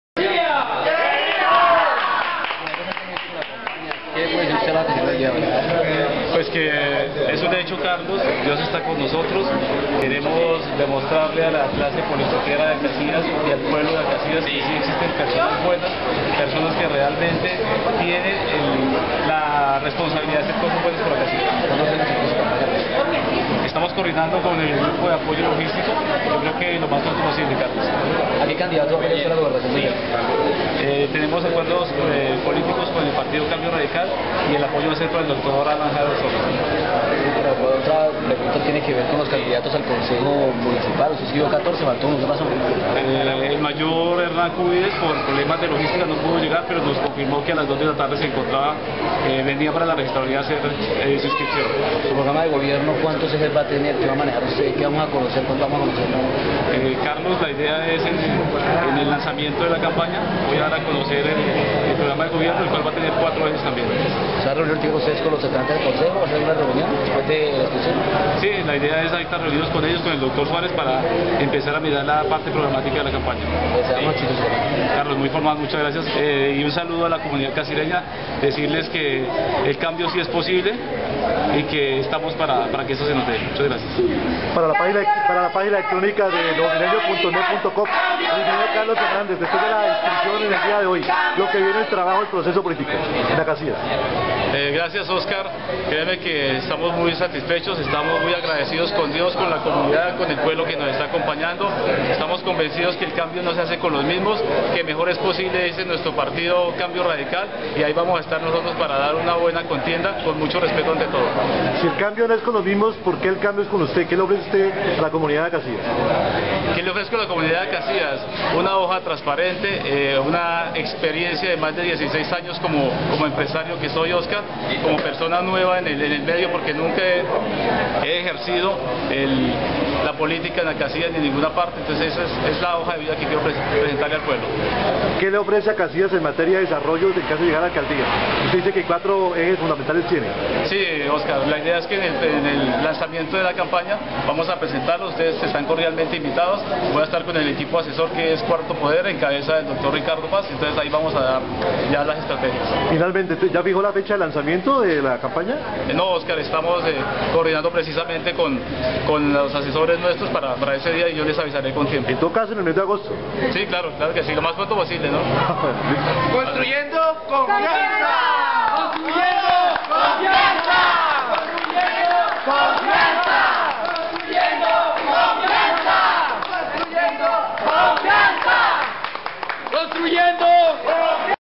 Carlos Hernández se inscribió a la Alcaldía
Es el candidato del partido Cambio Radical